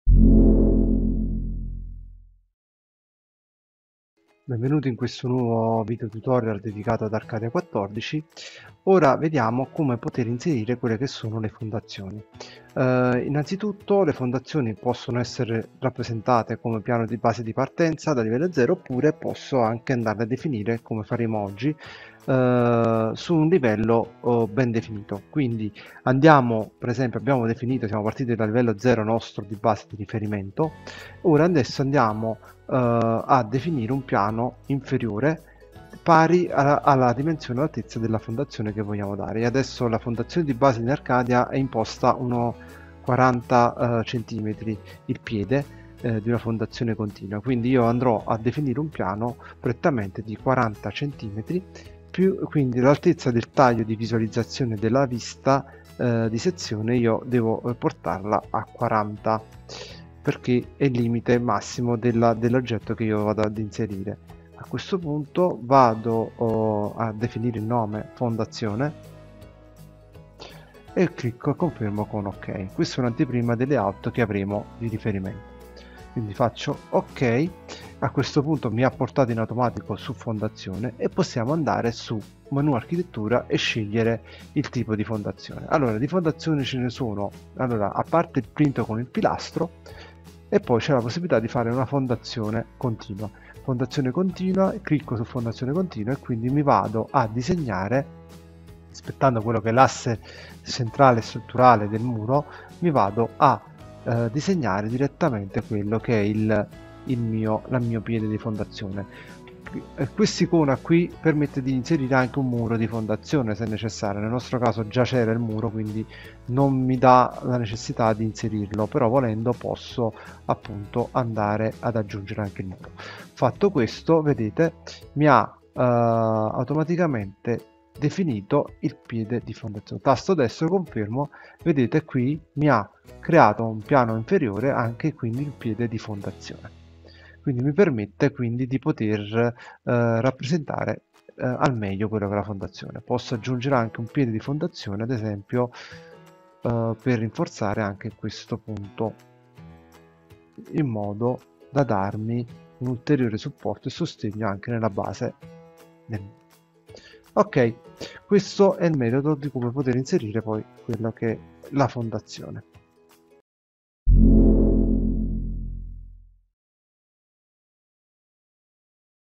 ArCADia BIM - Fondazioni
Come generare un piano di fondazione con ArCADia BIM.